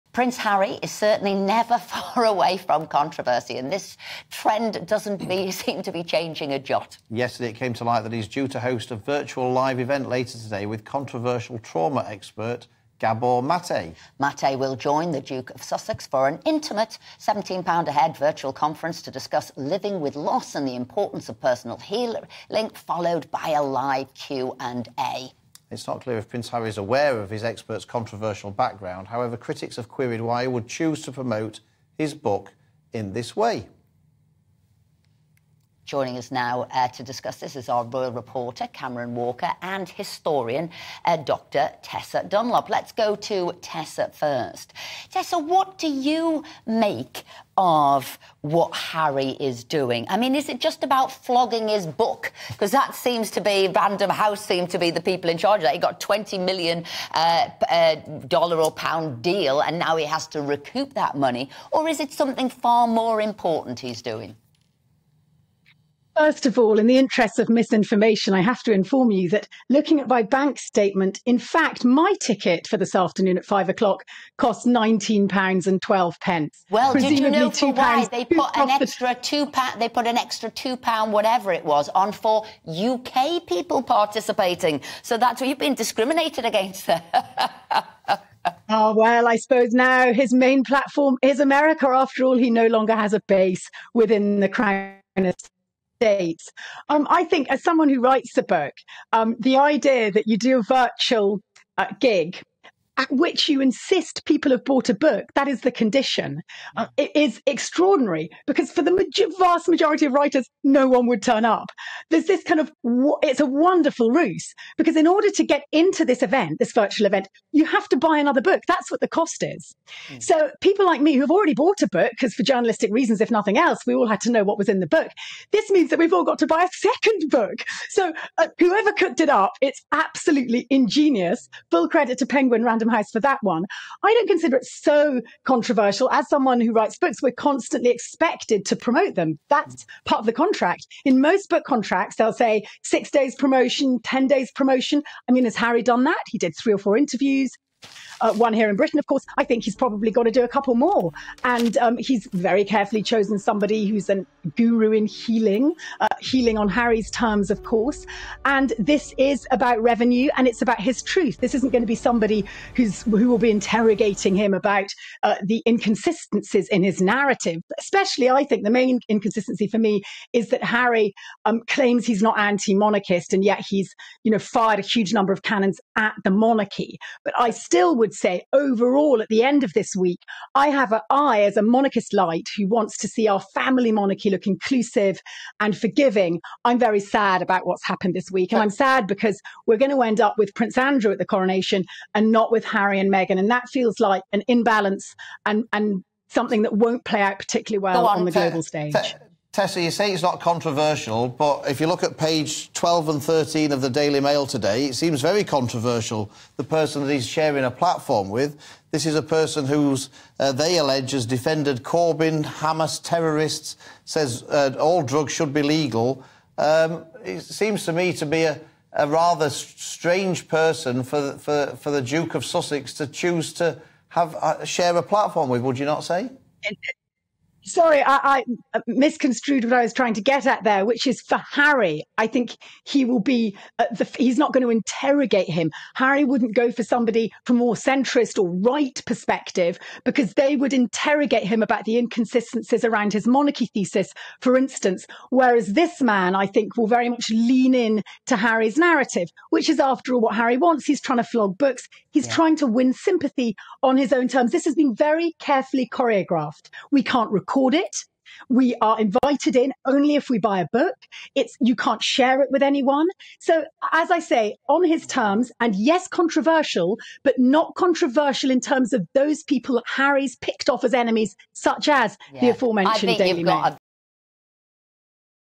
Prince Harry's intimate Q&A to promote 'Spare' detailed by Dr Tessa Dunlop
‘In order to get into this event you have to buy another book!’

Historian, Dr Tessa Dunlop details Prince Harry’s intimate Q&A to promote his memoir ‘Spare.'